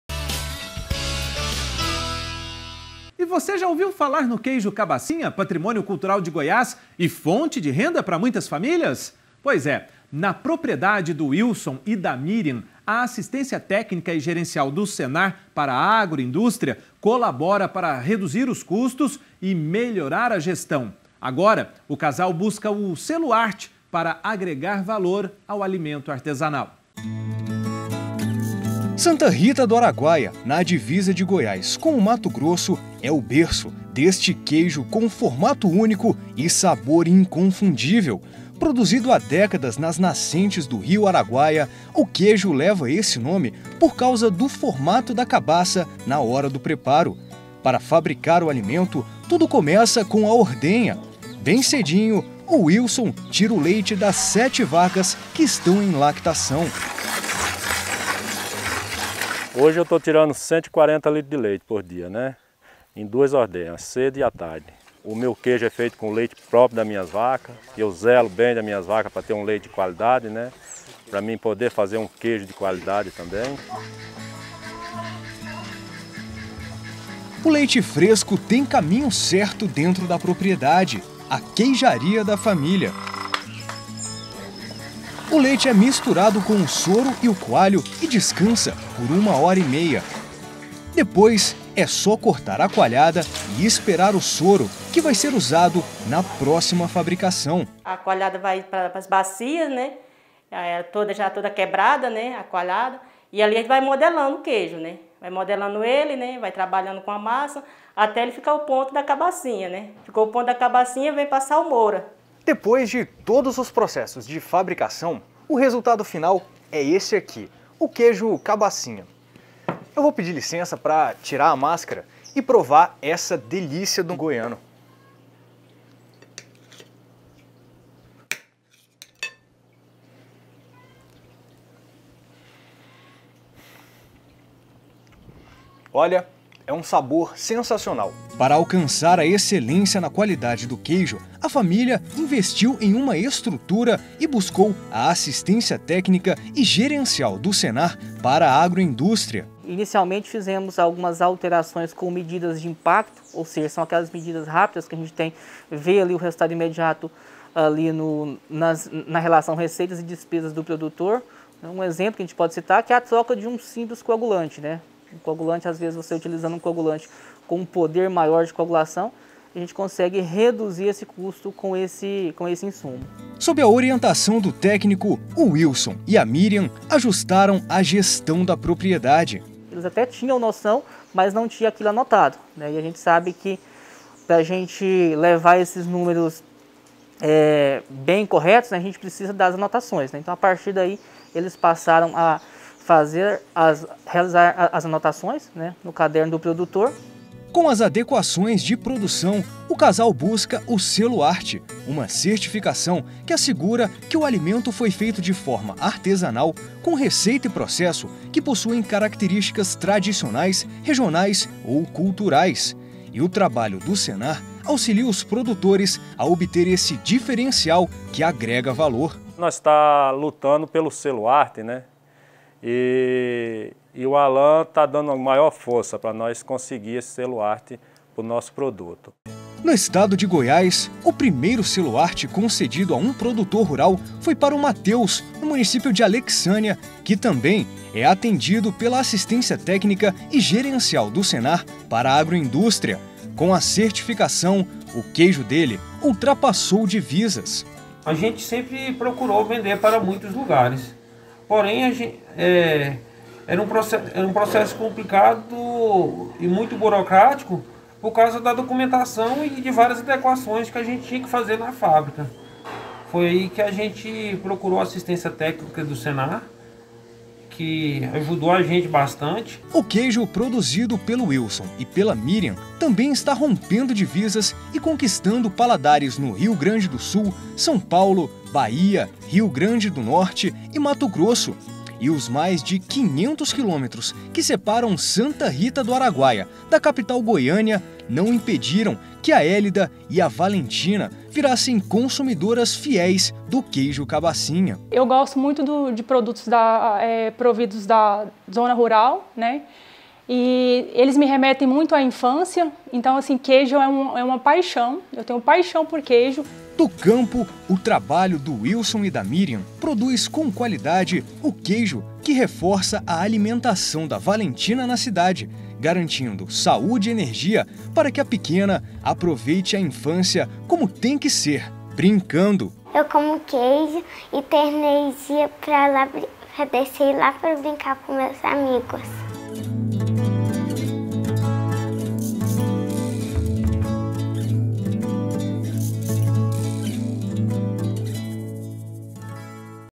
NOSSO AGRO - Queijo Cabacinha. ATeG do Senar GO para Agroindústria contribui para melhorar produção
E você já ouviu falar no queijo cabacinha, patrimônio cultural de Goiás e fonte de renda para muitas famílias? Na propriedade do Wilson e da Mirian, a Assistência Técnica e Gerencial do Senar para a Agroindústria colabora para reduzir os custos e melhorar a gestão. Agora, o casal busca o Selo ARTE, para agregar valor ao alimento artesanal.